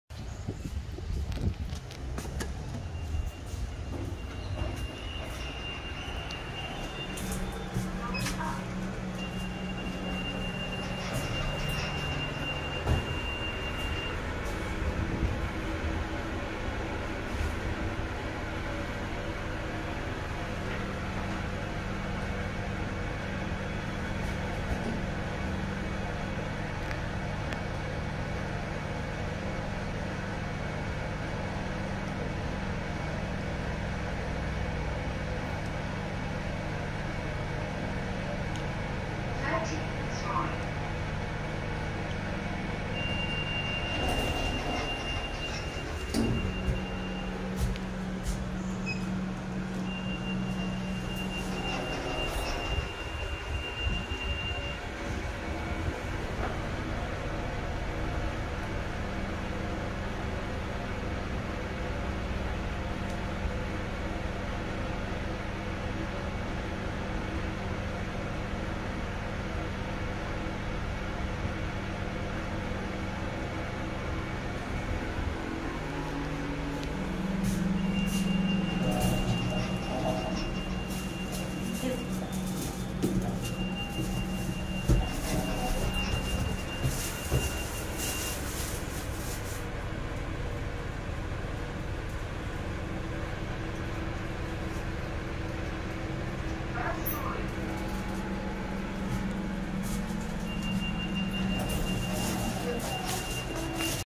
2008 Sigma Lift - Block 107 Bukit Panjang/Gangsa Road
15 Feb 2022

A chad Sigma lift at Gangsta Road.

The original Fujitecs here got replaced during IUP to serve all floors.